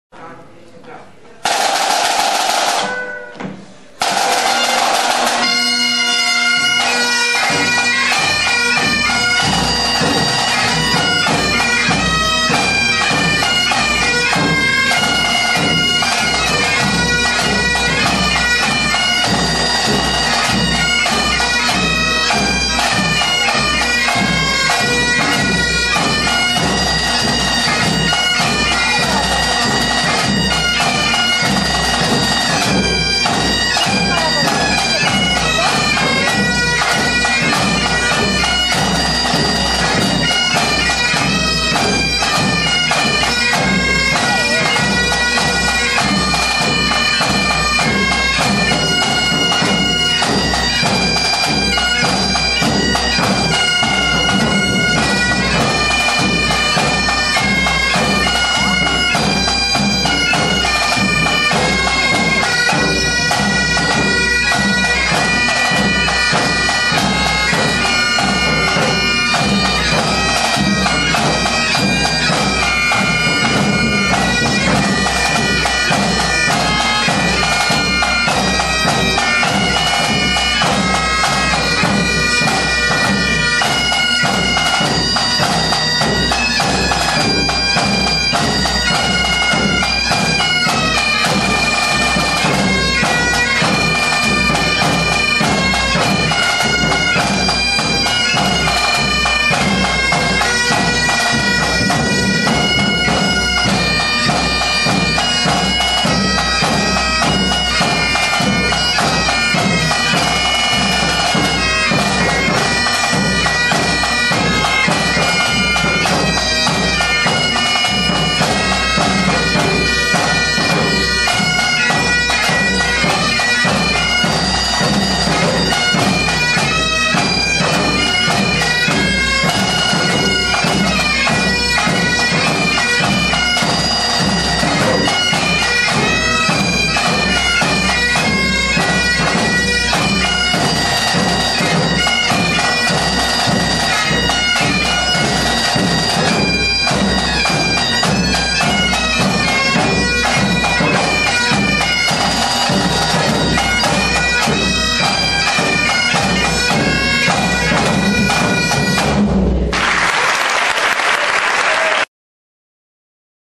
Cityof Barcelona Pipe Band
El grupo City of Barcelona Pipe Band, actuó en el encuentro internacional de gaitas del Centro Gallego de Barcelona 2012